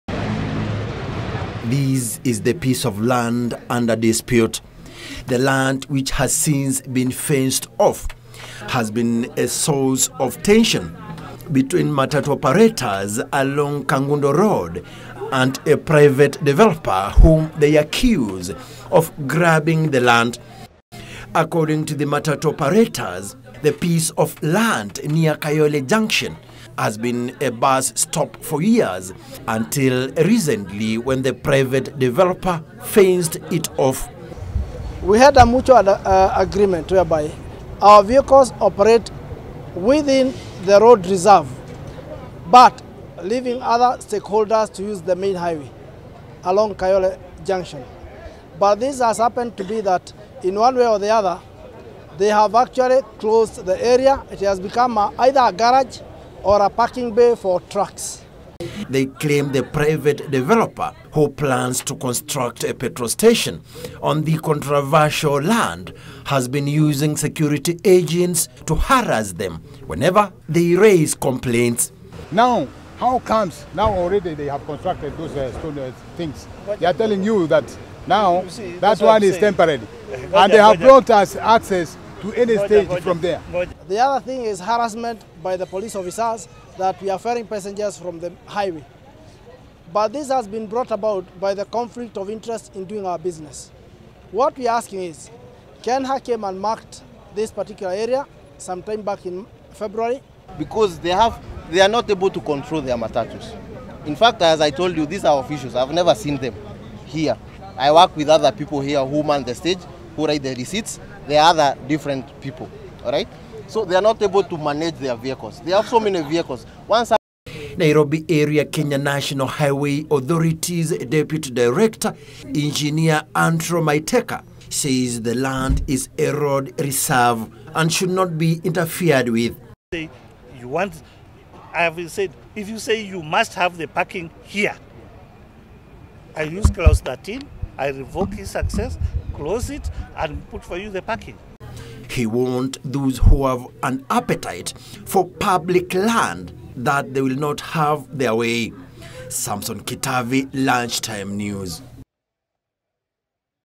Matatu operators plying Kangundo road are up in arms
Matatu operators plying  along Kangundo road are up in arms after a private developer allegedly fenced off a piece of land initially used as a bus stop near Kayole junction in Nairobi. The developer who plans to construct a petrol station at the disputed land is said to have used the police to evict the Matatu operators, and the Kenya National Highway Authorities says the land in question is a road reserve.